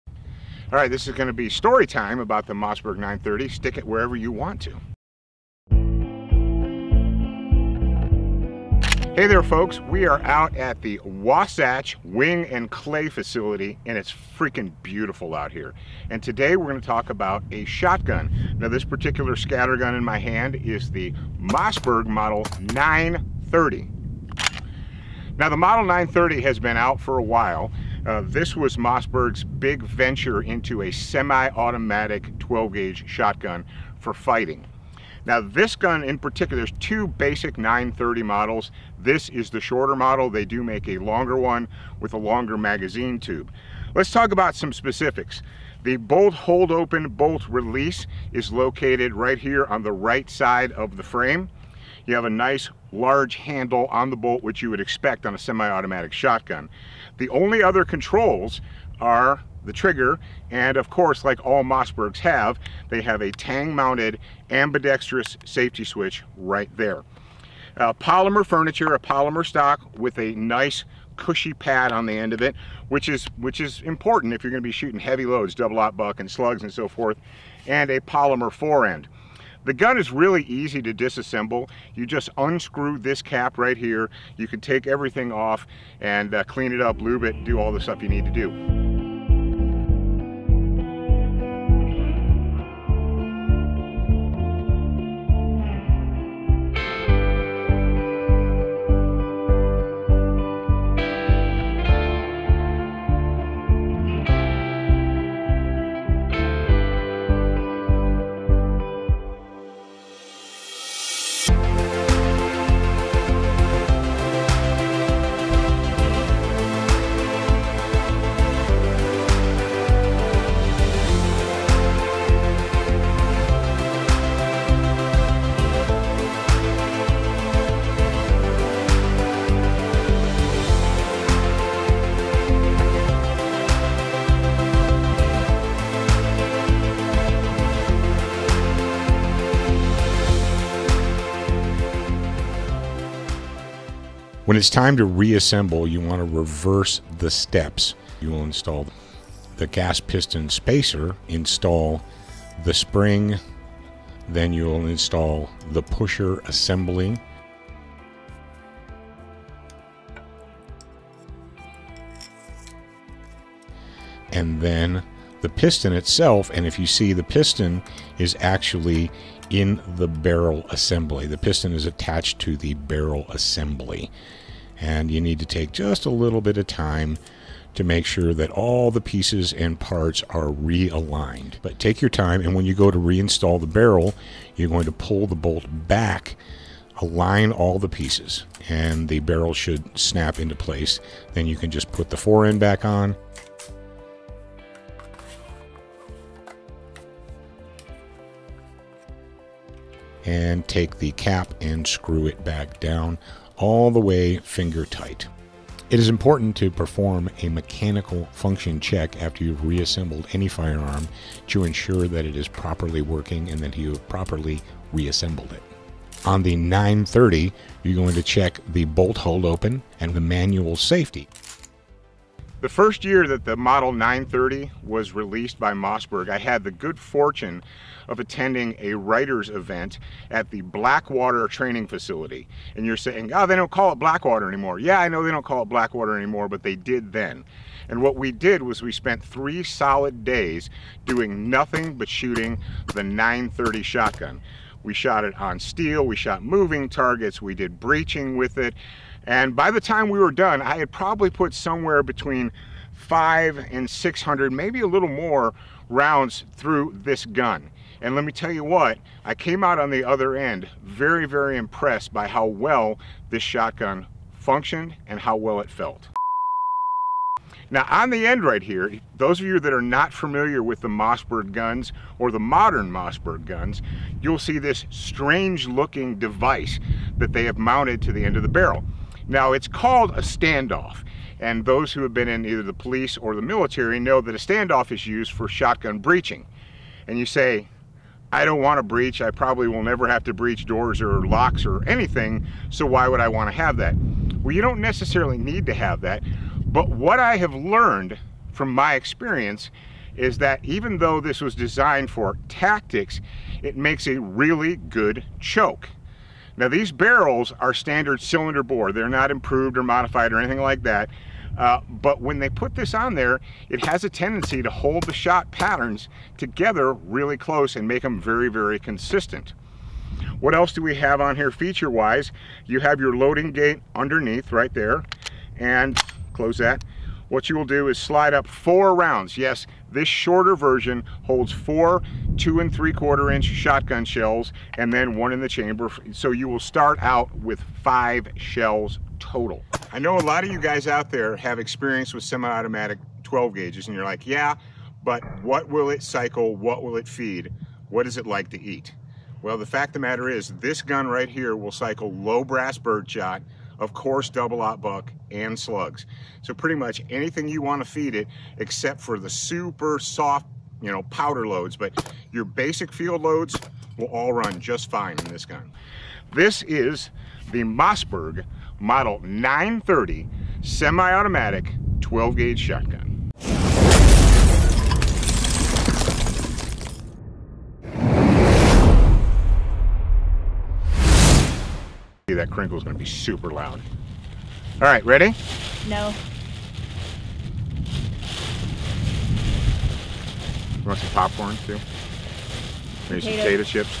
Mossberg 930 Review [Stick it wherever you want]
Everything you need (or wanted) to know about the Mossberg model 930 semi-automatic 12-gauge shotgun. Paul covers disassembly, reassembly, performing a function check, preferred food, and a funny story from the Blackwater days.